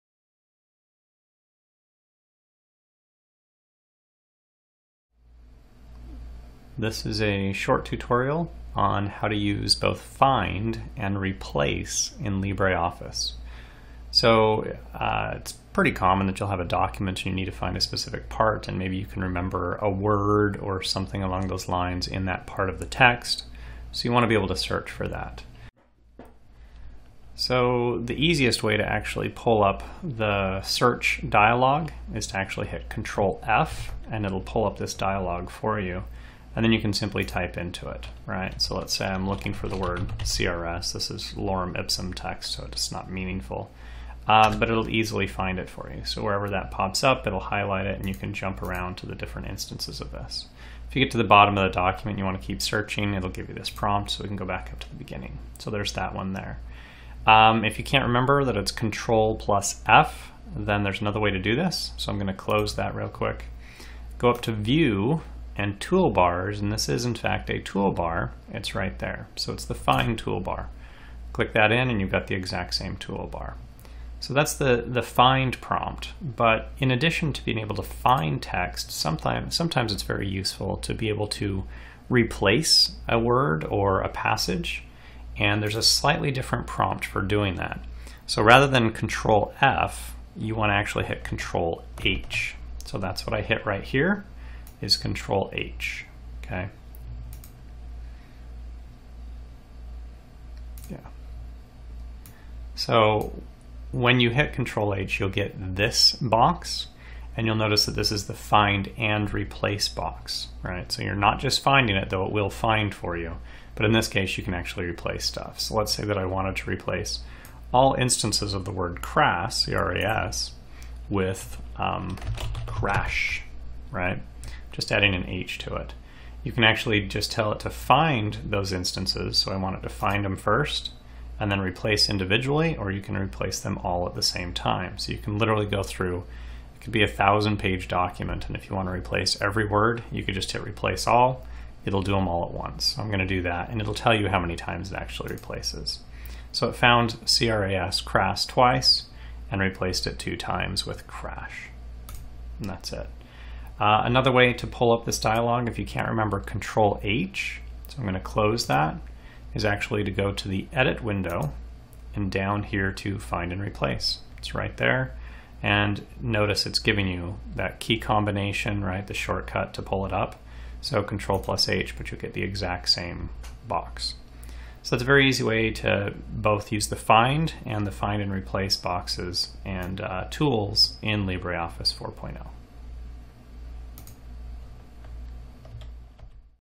LibreOffice 4 How To: use find and replace
This is a video tutorial on how to use the find and "find and replace" tools in LibreOffice 4.